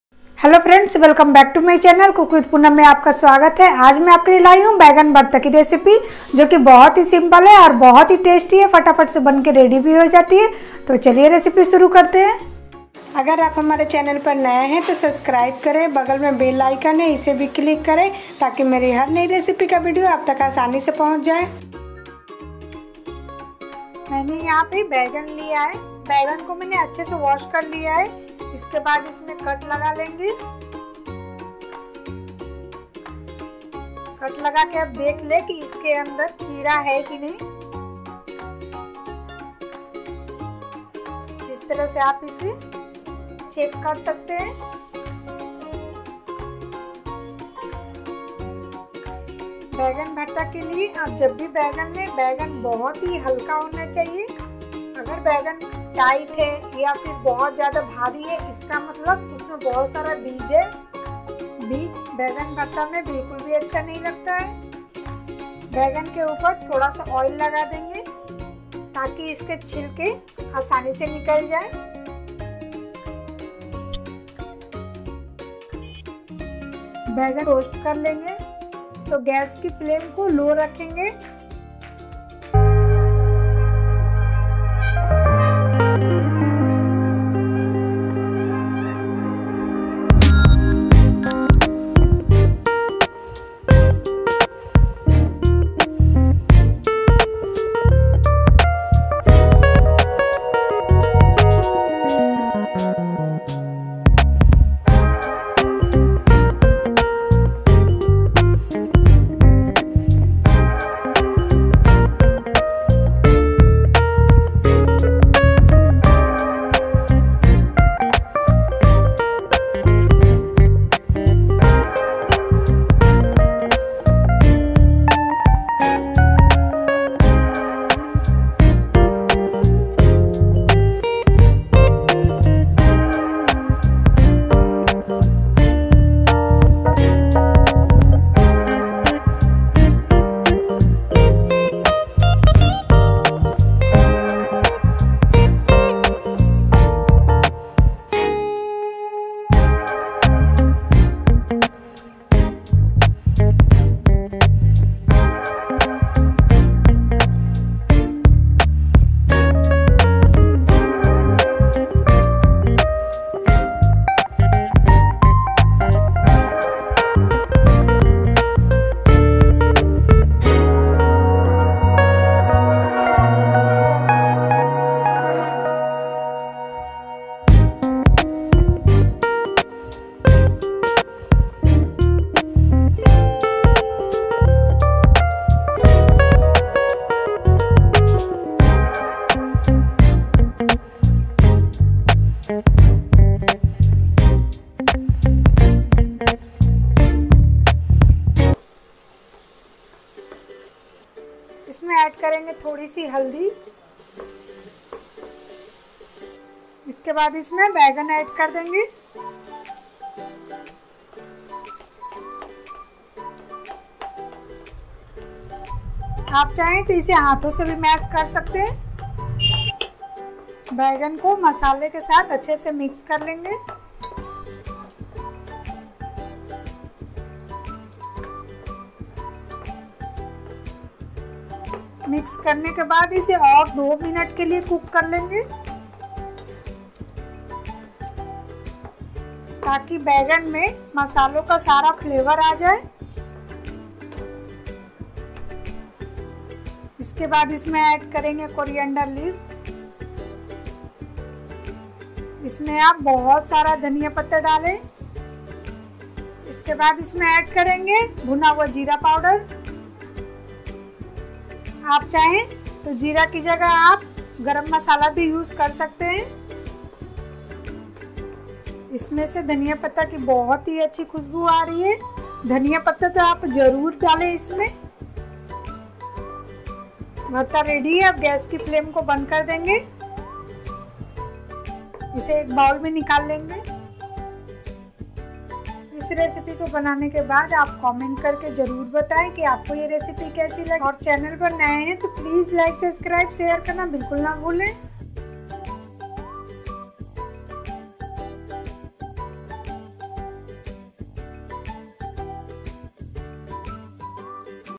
tasty baigan bharta recipe(बैगन चोखा रेसिपी)
is video mein mein baingan bharta banana sikha Rahi hoon